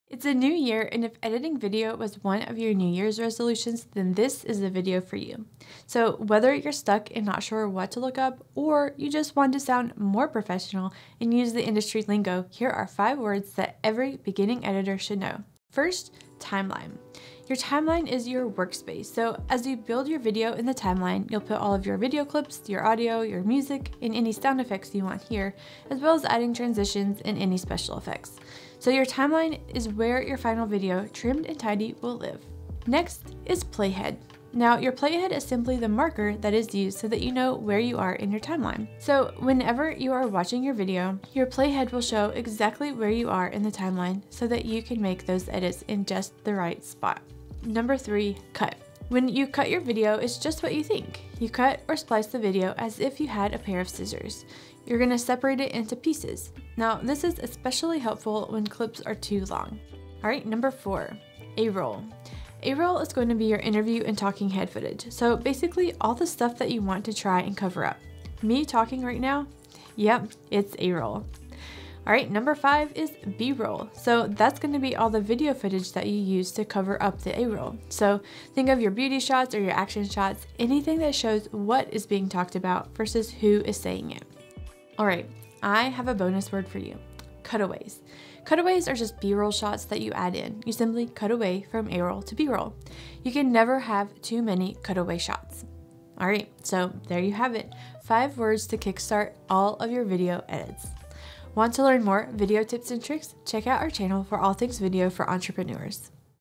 Video Editing Tips: 5 Keywords for Beginners
Want to talk AND edit like the pros? Check out today’s youtube video where I share 5 key words every beginning editor should know.

Want to learn more editing tips? This channel has got you covered with all things video for entrepreneurs.

Come say hi on Instagram @hereforgoodfilms